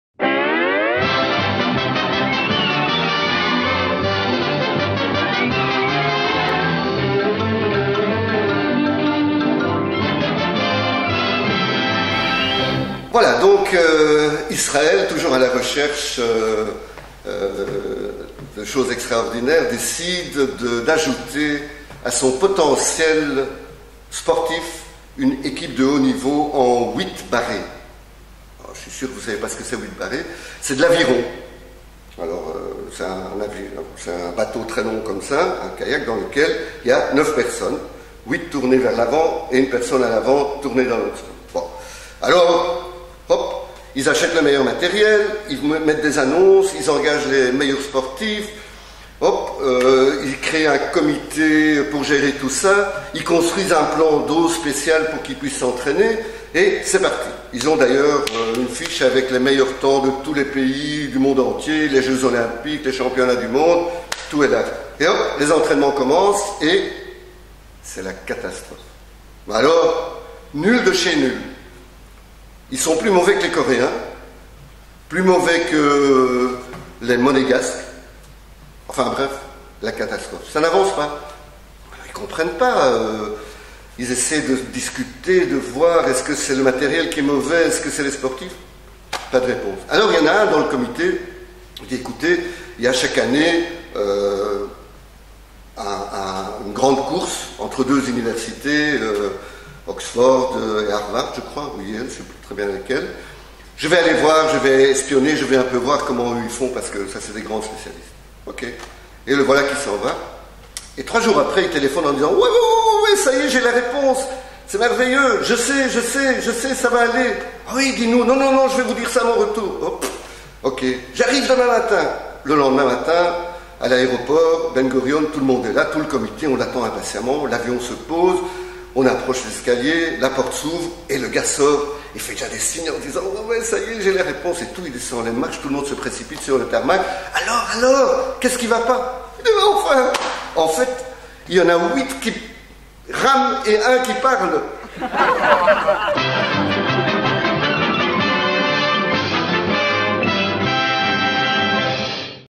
Humour juif - Le 8 barré
Blague juive contée par André Reinitz, enregistrée au Centre Communautaire Laïc Juif de Bruxelles en 2013.